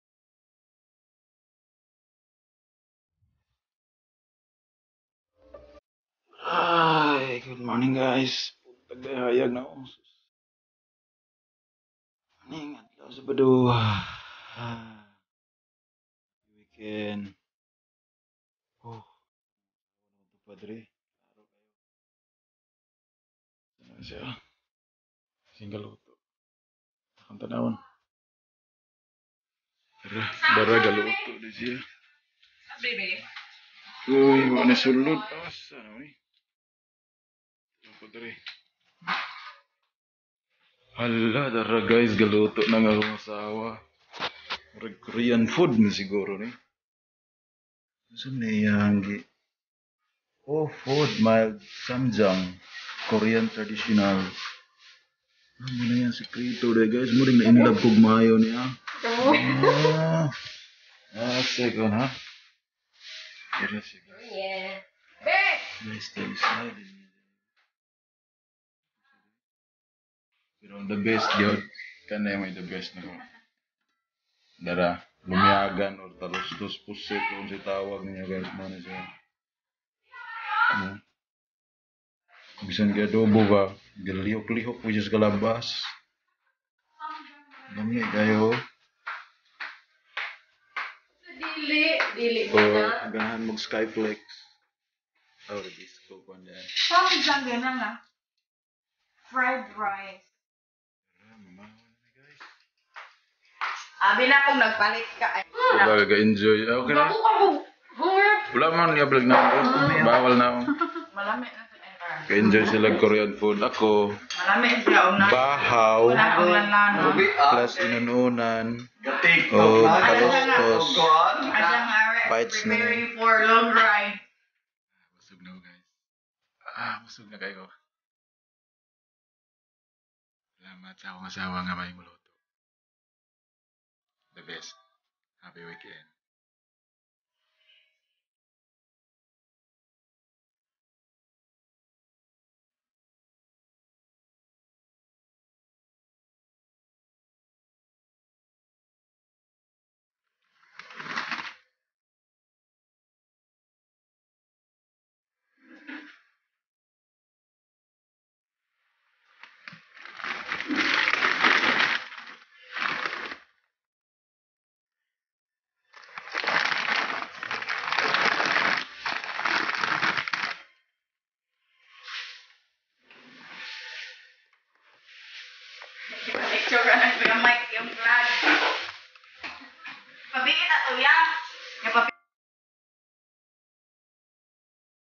Korean food and Filipino food for breakfast
Good food is the foundation for genuine happiness. - Auguste Escoffier

Pero mas masarap parin ang Pinoy na luto.

Happy breakfast 🍳